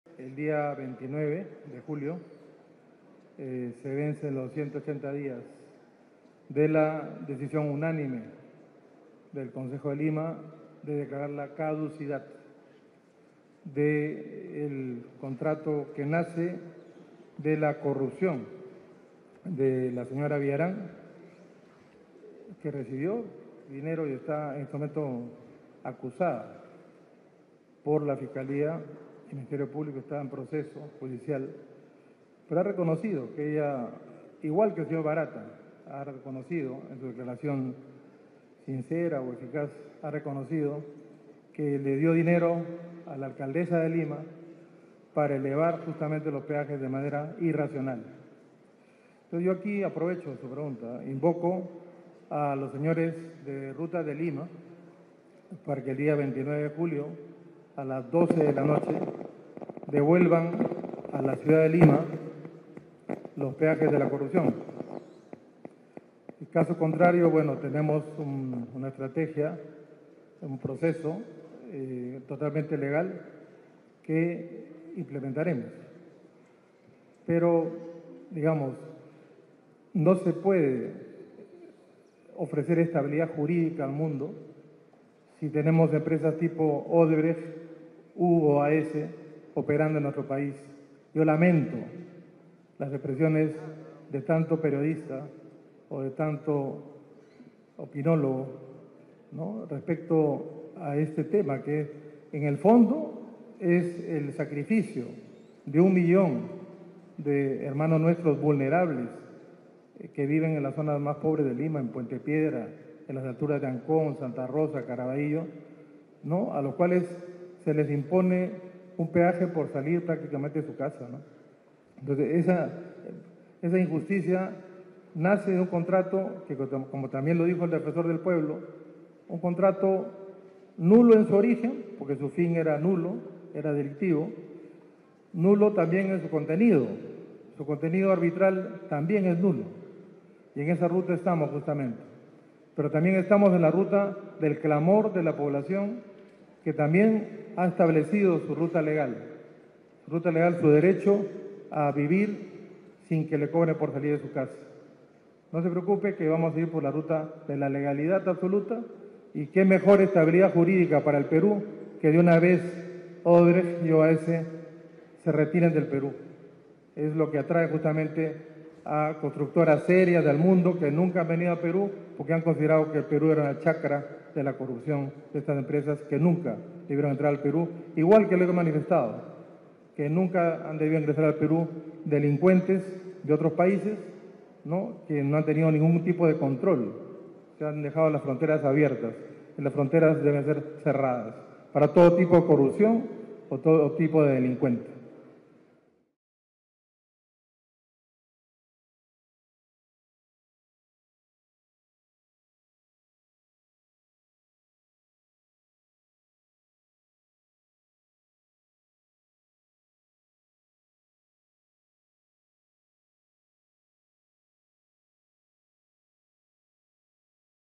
Rafael LÓPEZ ALIAGA solicitó a RUTAS DE LIMA "DEVOLVER los PEAJES" hasta el 29 de julio
El alcalde de Lima, Rafael López Aliaga, dio como plazo máximo el sábado 29 de julio para que Rutas de Lima "devuelva a la ciudad los peajes de la población". Como se recuerda, el Concejo Metropolitano aprobó romper el contrato con la empresa en enero de este año por acusaciones de corrupción hacia la concesionaria.